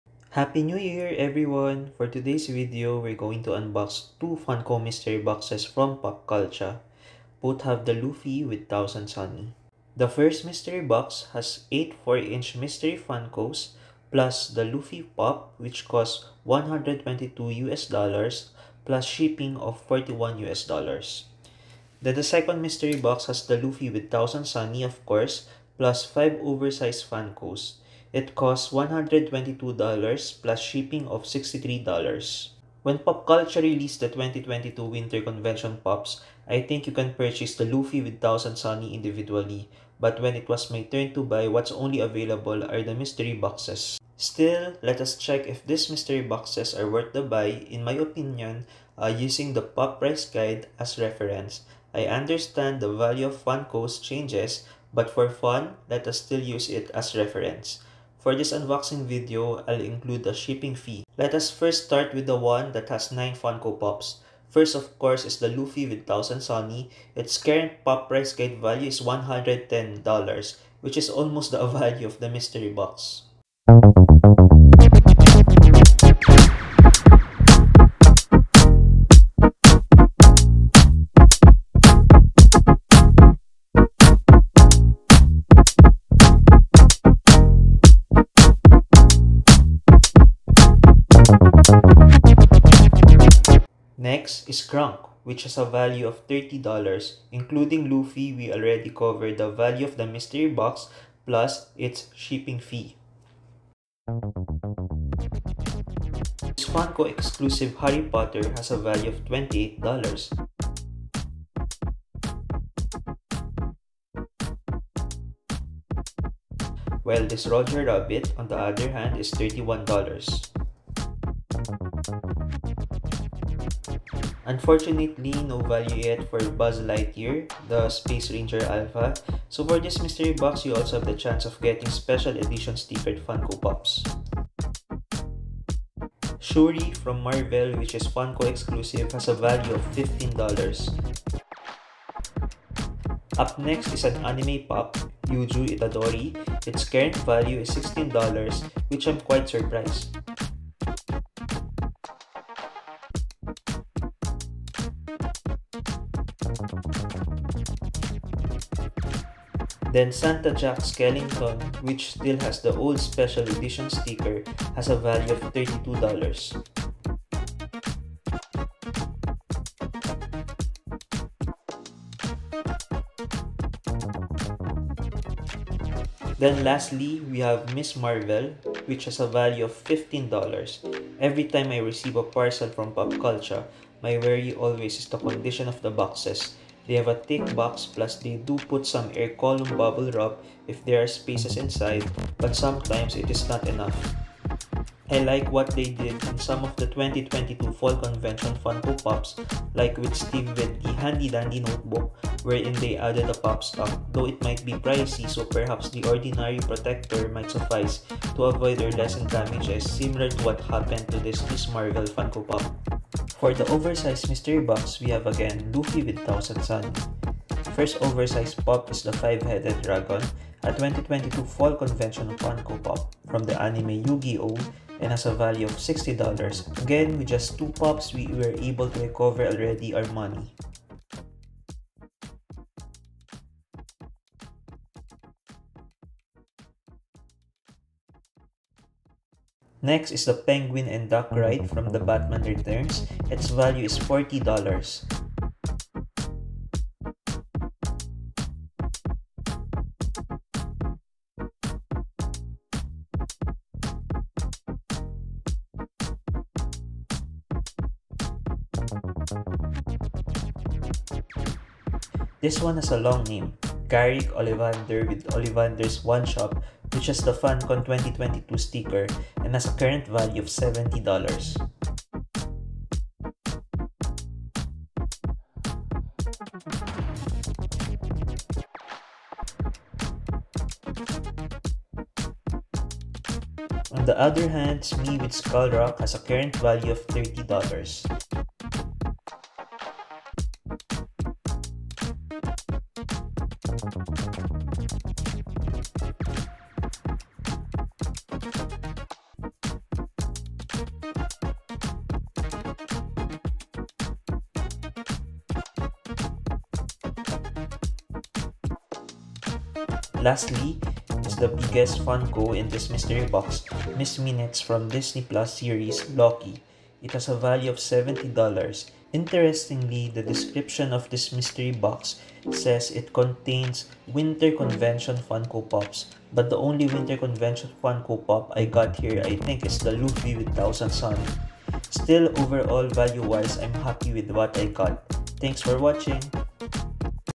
Popcultcha Luffy with Thousand Sunny Funko Pop! Mystery Boxes!
Unboxing 2 Funko mystery boxes I purchased from Popcultcha. Both mystery boxes contain the Luffy with Thousand Sunny Funko Pop!

The first mystery box contains 8 regular sized exclusive Funko Pop! (excluded in the count is the Luffy with Thousand Sunny Funko Pop!).

The second mystery box contains 5 oversized exclusive Funko Pop (excluded in the count is the Luffy with Thousand Sunny Funko Pop!).

Using the Pop Price Guide as reference, let us see if we get the value of the money we paid.